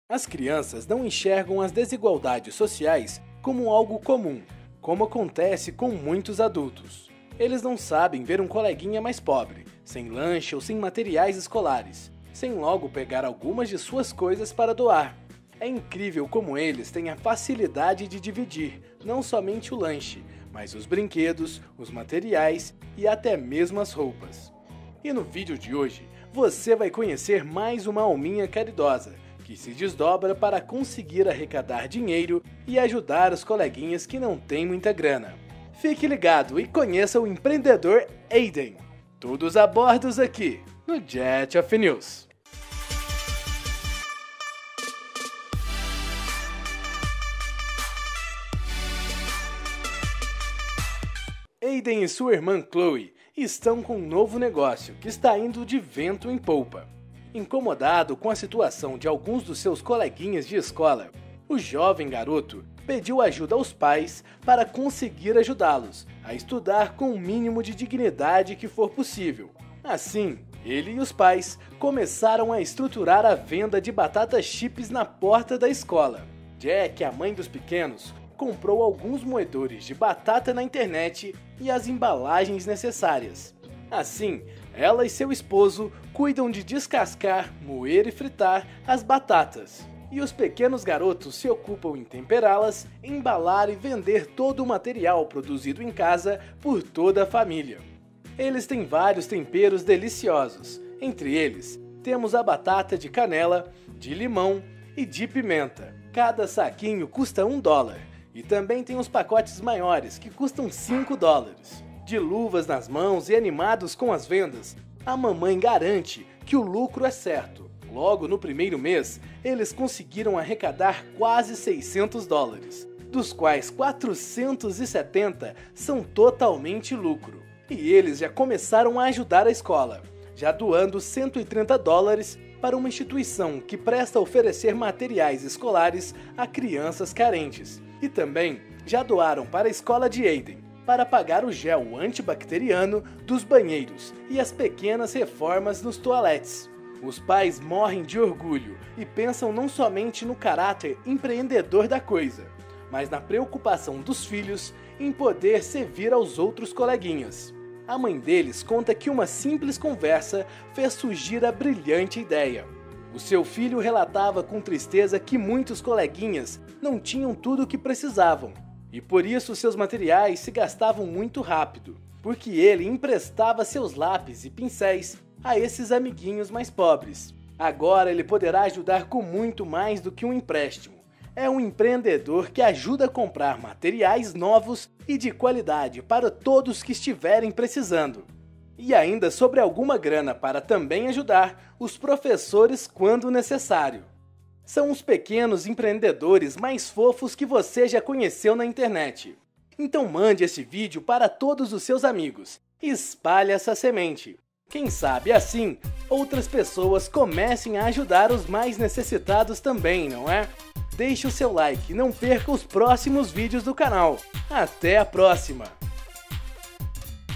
Imãos iniciam negócio de batata frita para ajudar colegas de escola
Roteiro:     Silas Paixão                 @silaspaixao_
Edição:      Jefferson Levy            @levy_jefferson
Locução:   Jay Neto                      @netojay